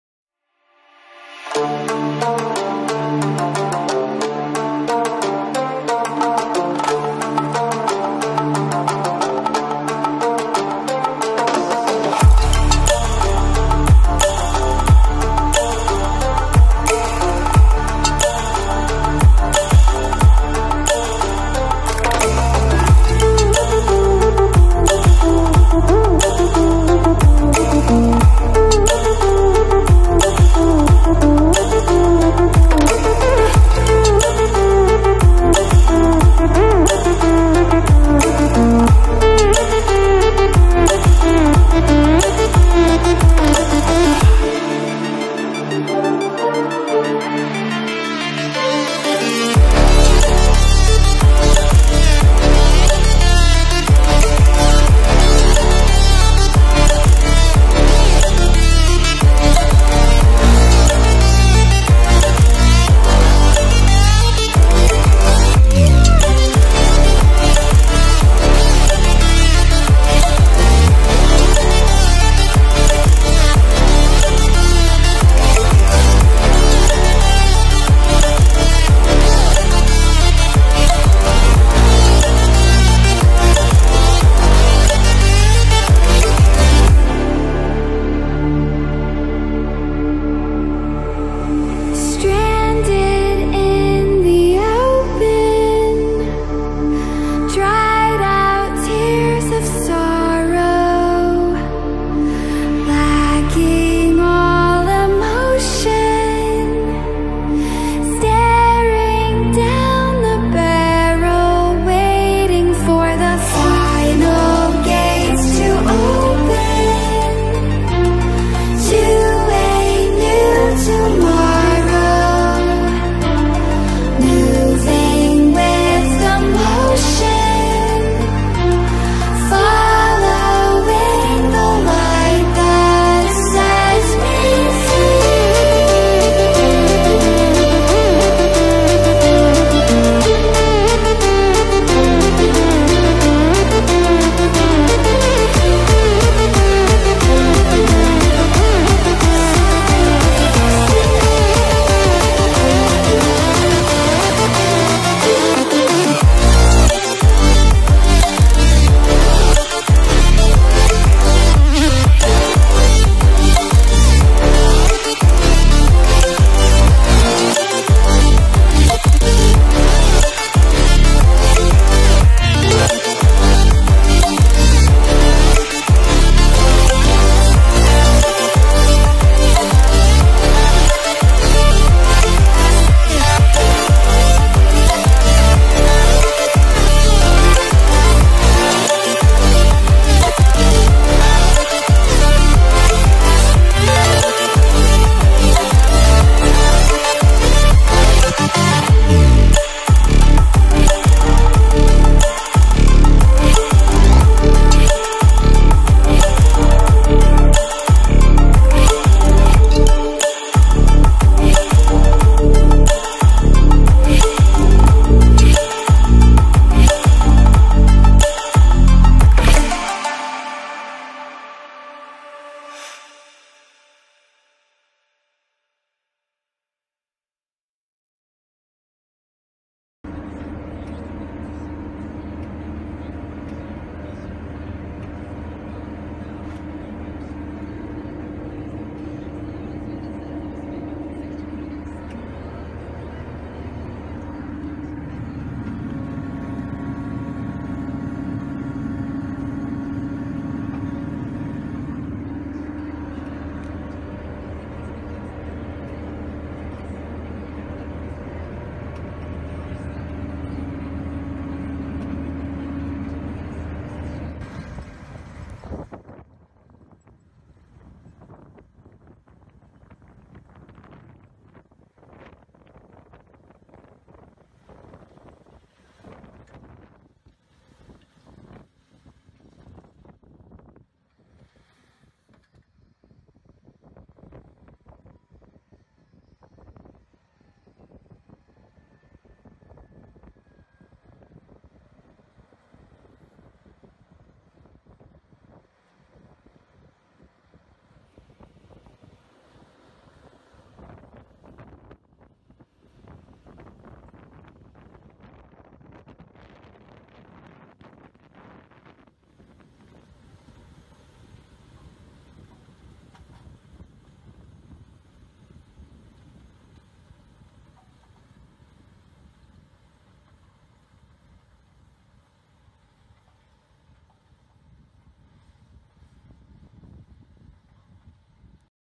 Drilling Spring Barley with the JCB Fastrac 4220
Bit of a windy day today but more videos to come 👍